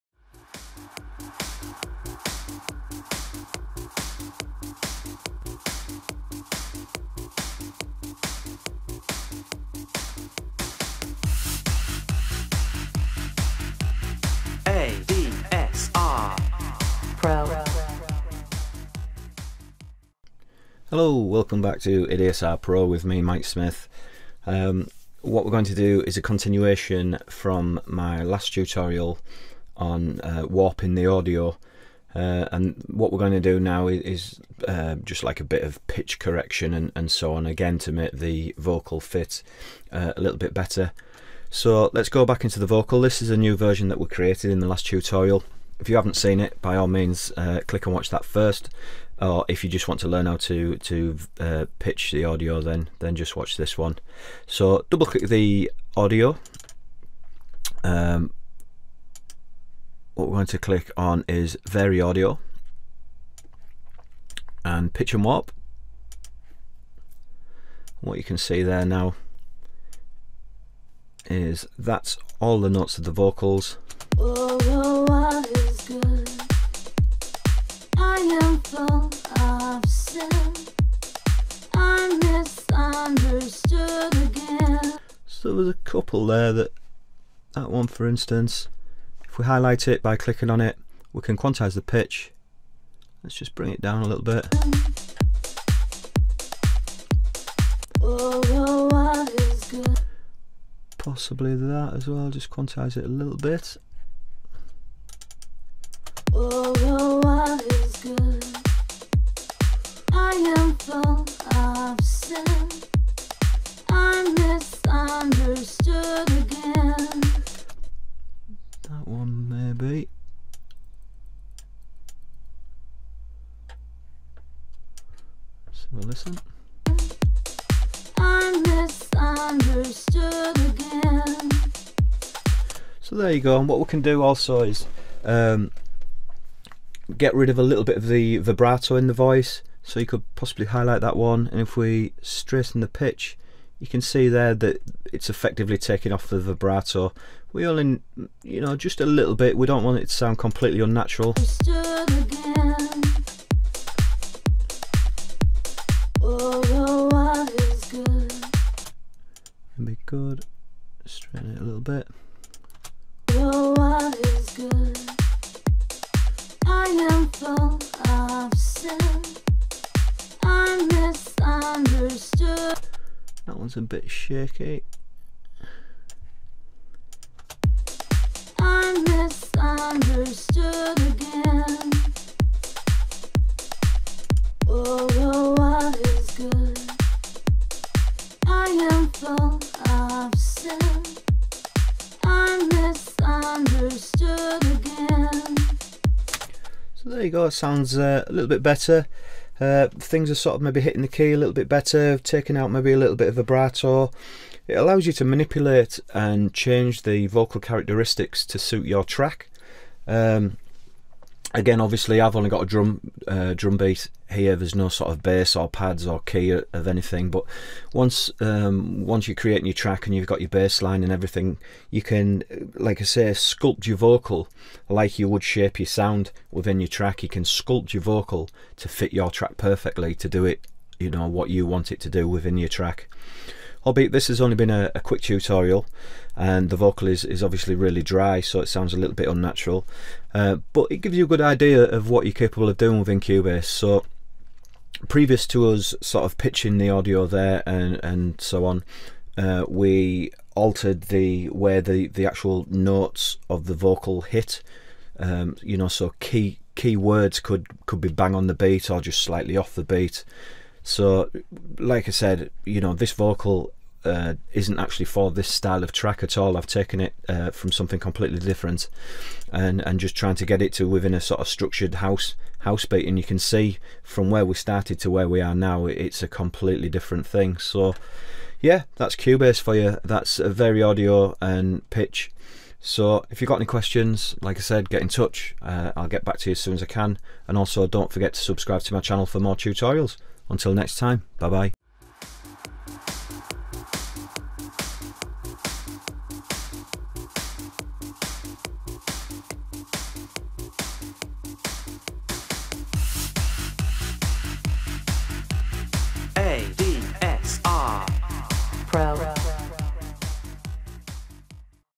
ADSR Pro using VariAudio in Cubase for pitch correction and vocal manipulation
A follow on video from warping vocals in Cubase, this time I show you how to pitch correct and get rid or tame down too much vibrato within the vocal. Another great built in tool from Steinberg Cubase.

Keep up to date with the latest tutorials from ADSR Pro by following our feeds: